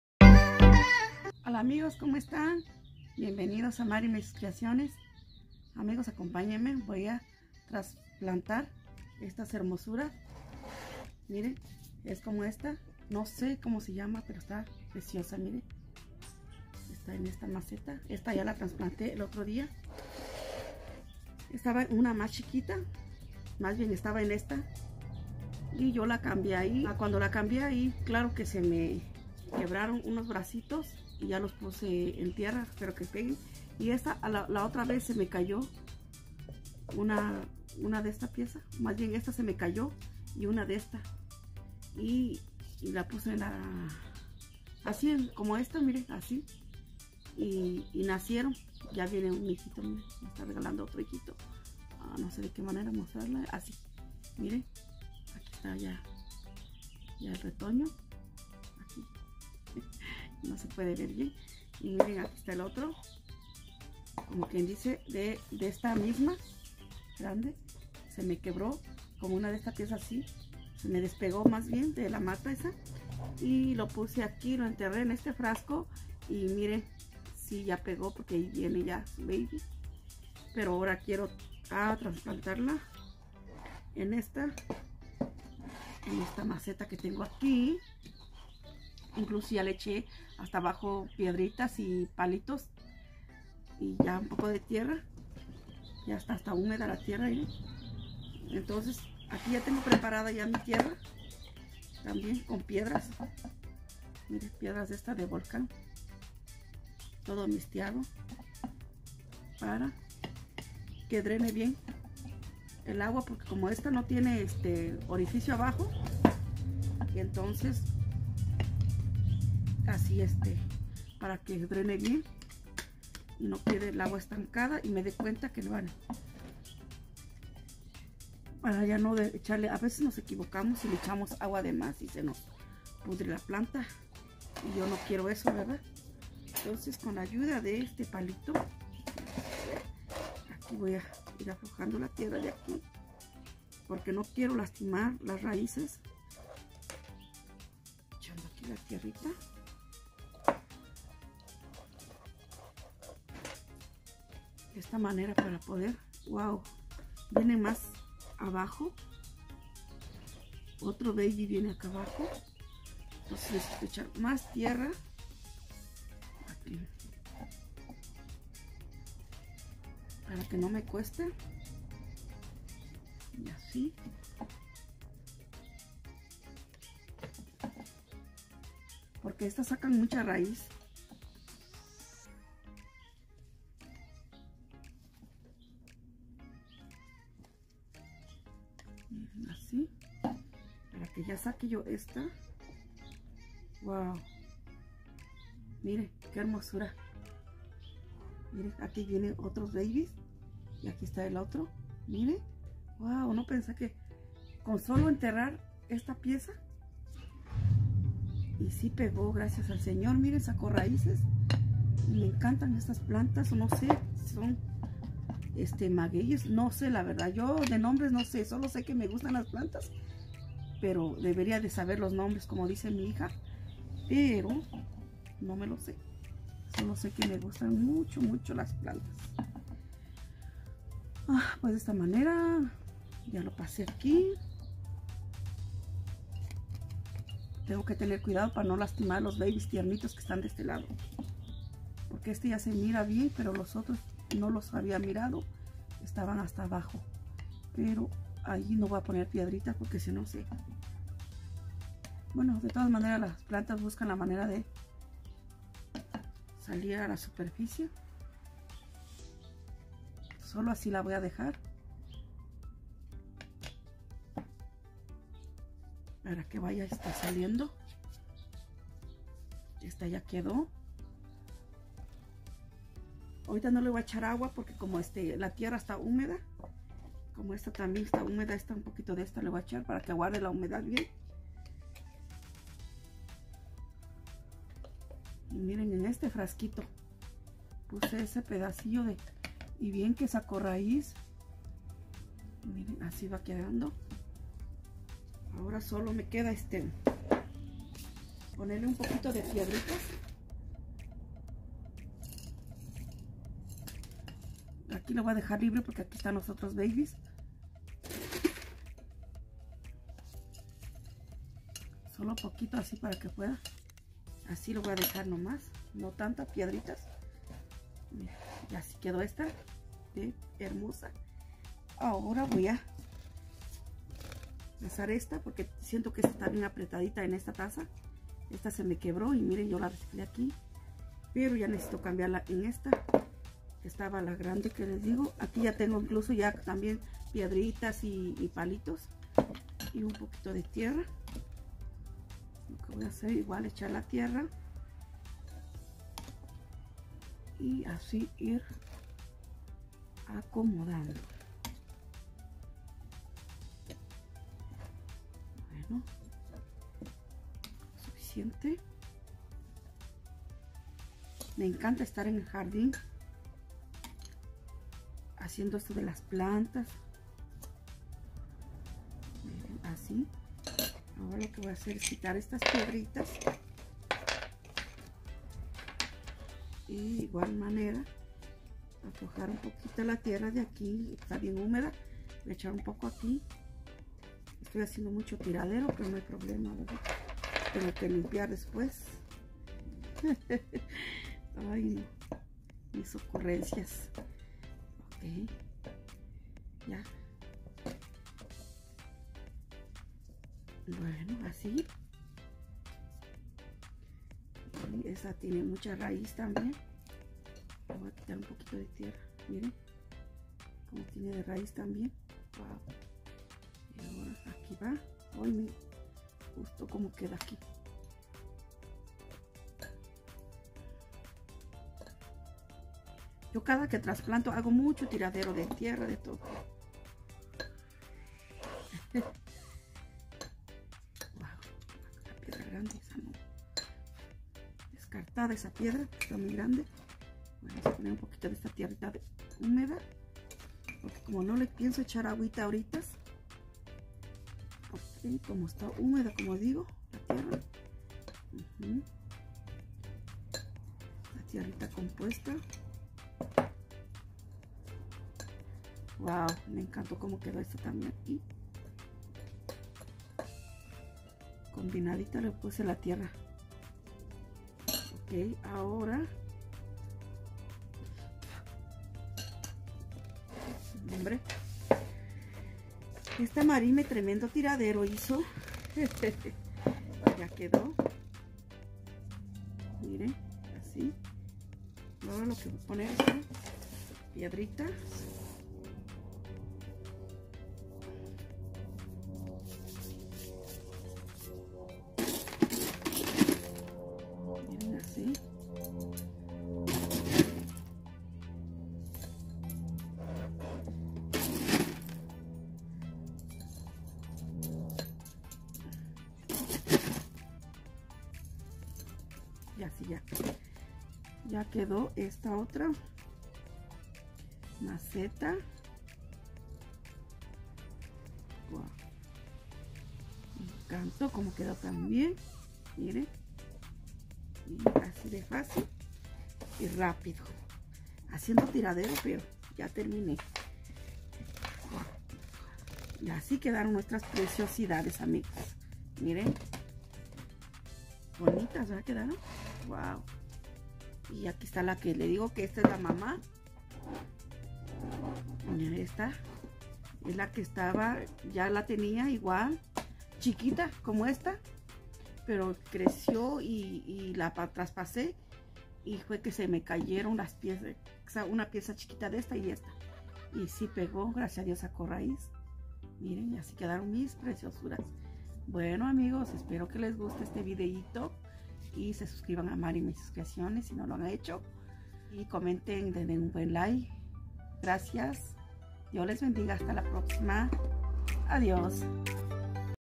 Trasplantado Dos Pedazos de mi Suculenta!🪴 | Maryme Y Sus Creaciones
Hola amig@s aqui con otro video de mis plantas!  hoy trasplante dos pedazos de una de mis hermosas suculentas grandes a dos macetas!  me gusto mucho como me quedaron espero que le guste! 🥰